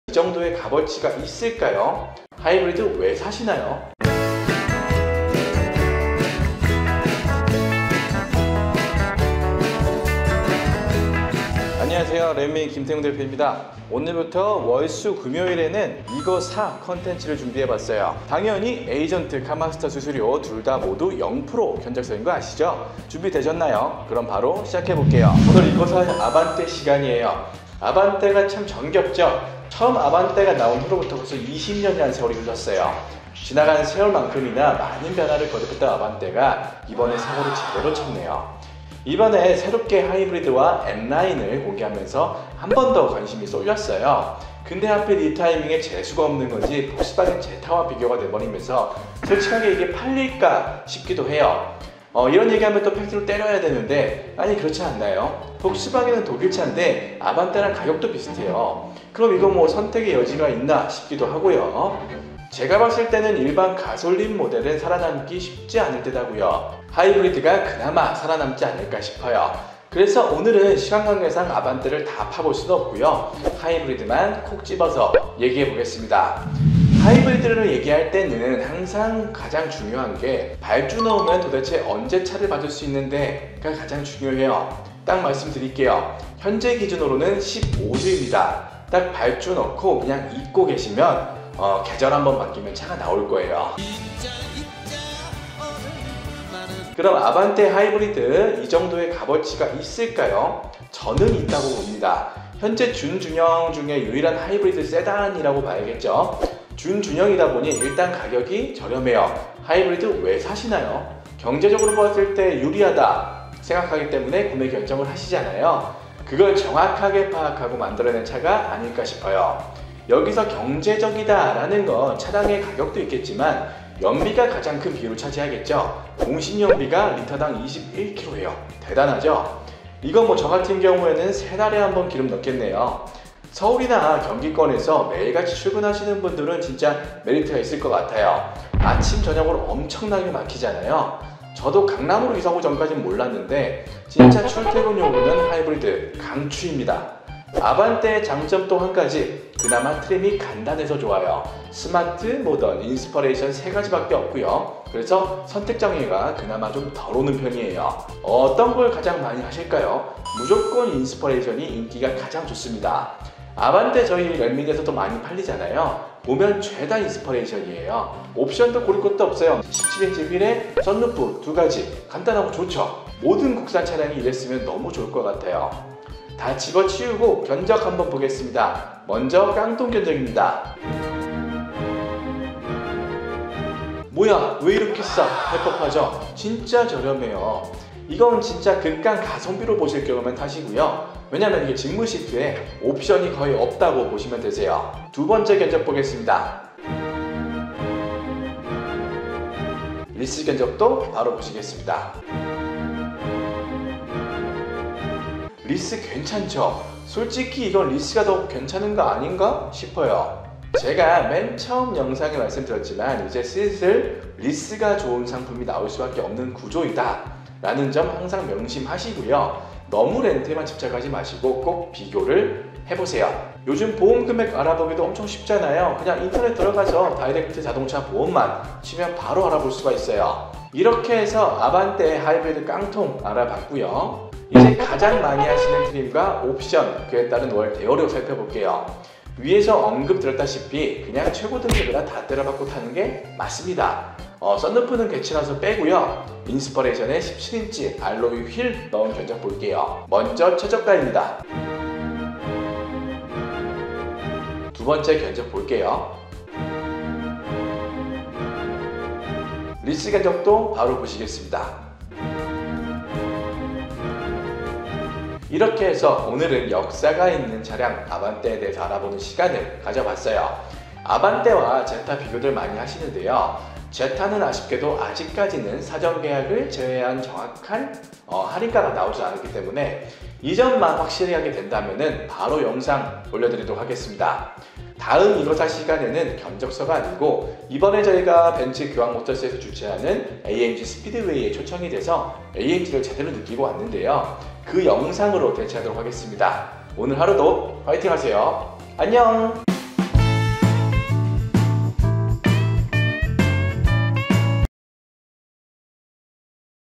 아반떼 하이브리드! 이거사! 공식연비 21km 출퇴근용 최강스펙! 가성비 찾는다면 렌미인에서 강력추천!
안녕하세요! 렌미인입니다.
오늘 이거사 주인공은 아반떼 하이브리드 입니다.

00:25 영상시작
03:01 깡통 최저가
03:16  깡통 승인잘나는  최저가
03:22 깡통 리스 견적서
04:11 추천차량 최저가
04:16 추천차량 승인잘나는 최저가
04:22 추천차량 리스 견적서